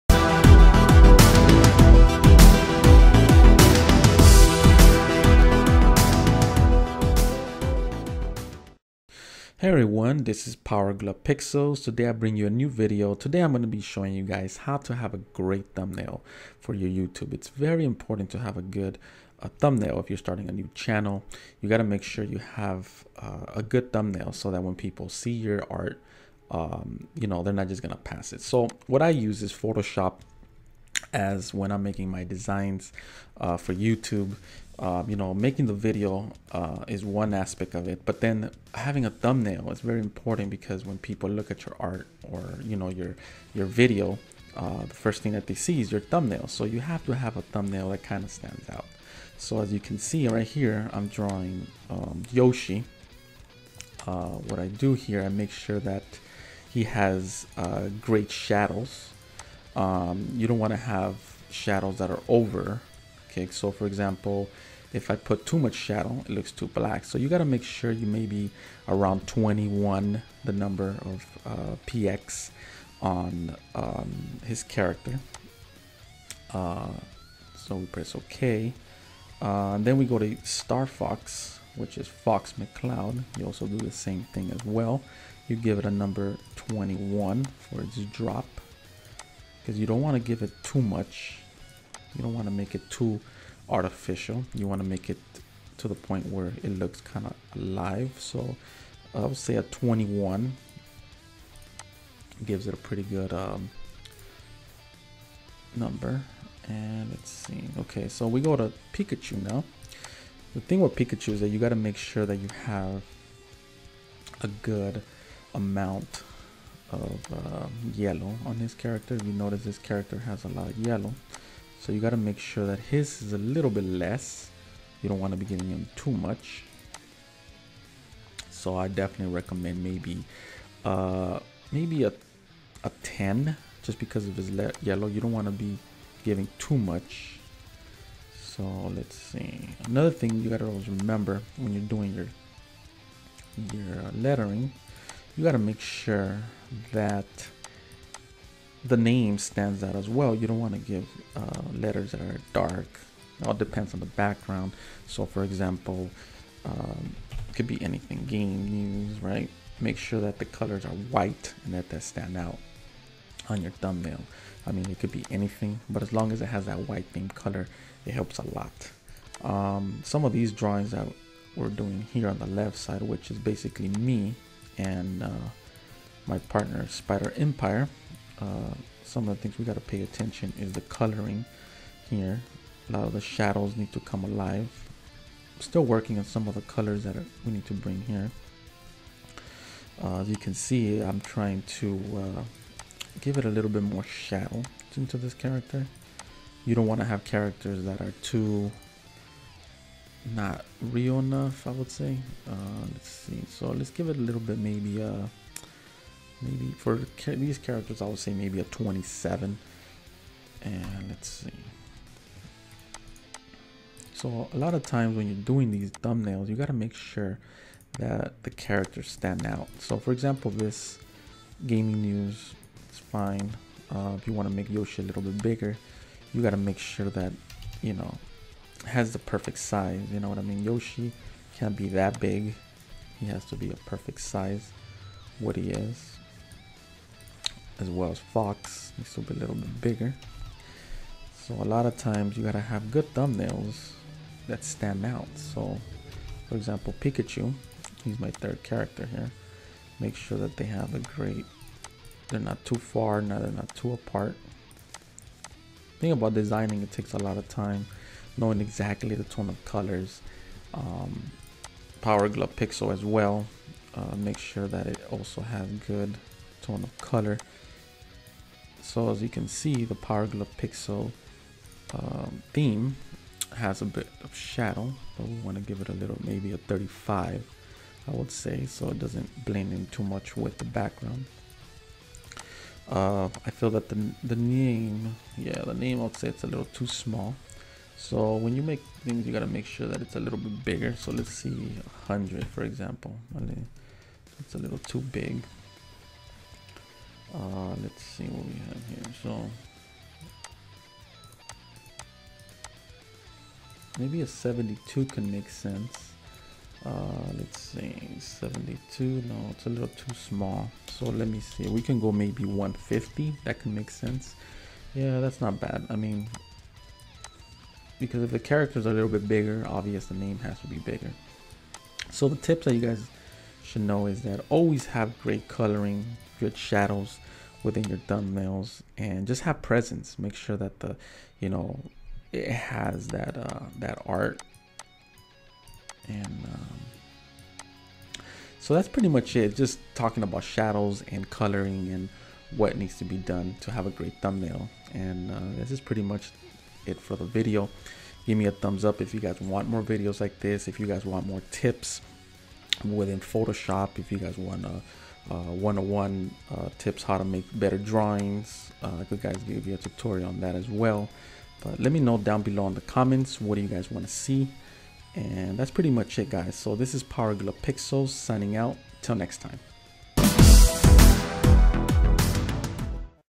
How to make a youtube thumbnail
this is pretty much is tutorial how to do a better thumbnails for youtube
this is a new series 101 tips let me know if you guys want more tips down below